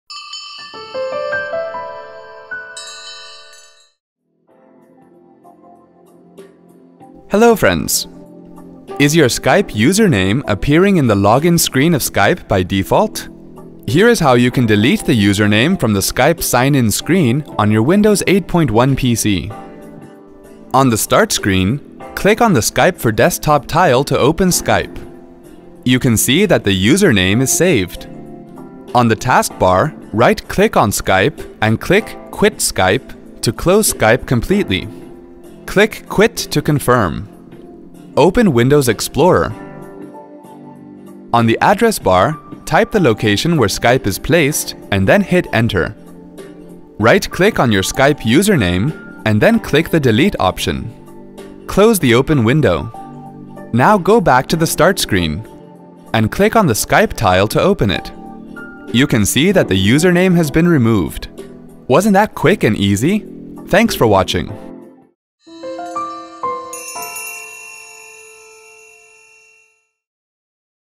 Remove username from sign-in window in Skype® for Windows® Desktop in Windows® 8.1
Is your Skype® username appearing in the login screen of Skype® by default? Follow the easy steps shown in this video to remove the username from the sign-in window in Skype® on a Windows® 8.1-based PC.